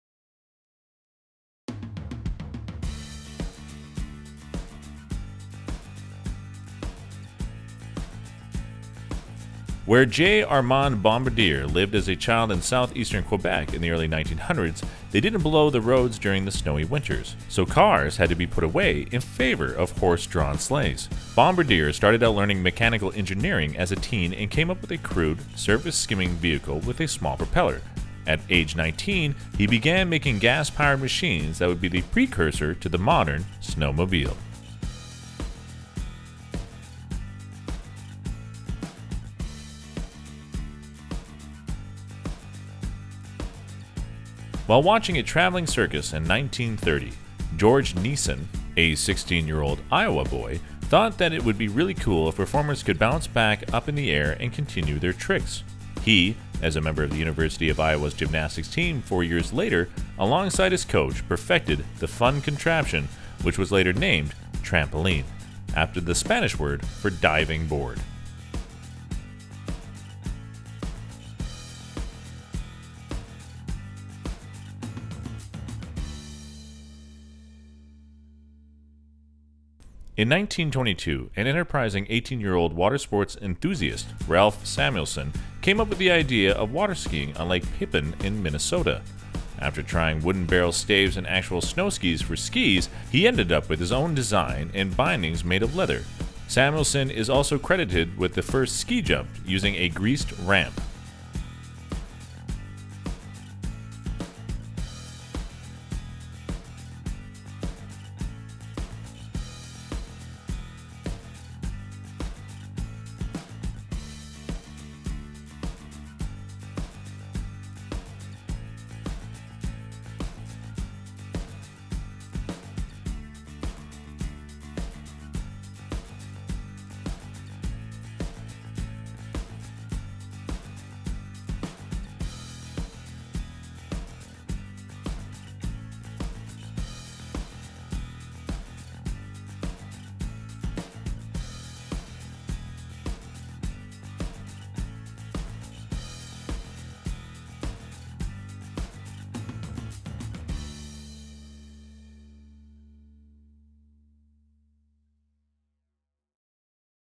Brilliant Inventions thought up by Children and Teens
Modern technology develops at incredible speeds and it is sometimes hard to keep track of new inventions and technologies. But one thing remains the same – most people think that the best inventions and breakthrough ideas belong to celebrated scientists and researchers.

However, it is not always necessarily true. There are brilliant ideas, which were turned intro great inventions by kids, teenagers and young scientists.

In our video we have reviewed the works of such young inventors:

- water-skis;

- snowmobile;

- trampoline.